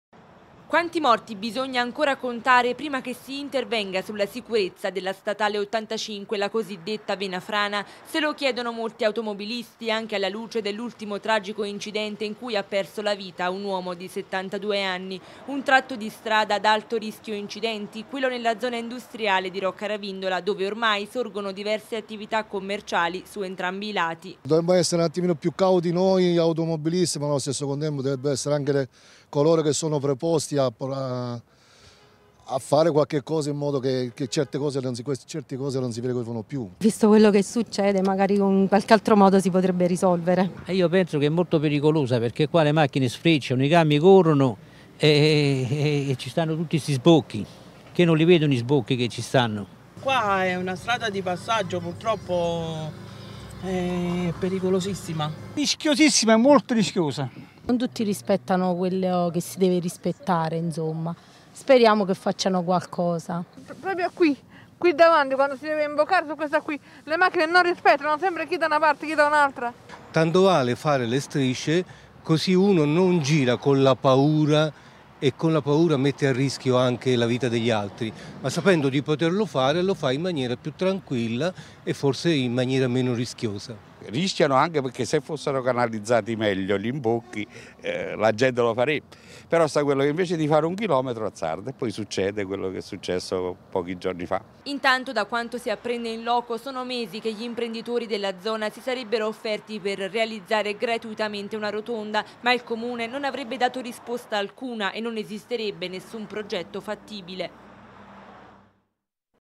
Venafrana, automobilisti chiedono interventi per rendere la statale sicura
Chiedono soluzioni immediate gli automobilisti che percorrono la Statale 85, la Venafrana. Alla luce del tragico incidente mortale di qualche giorno fa, siamo tornati nella zona industriale di Roccaravindola per ascoltare il grido d'allarme di chi percorre quel tratto di strada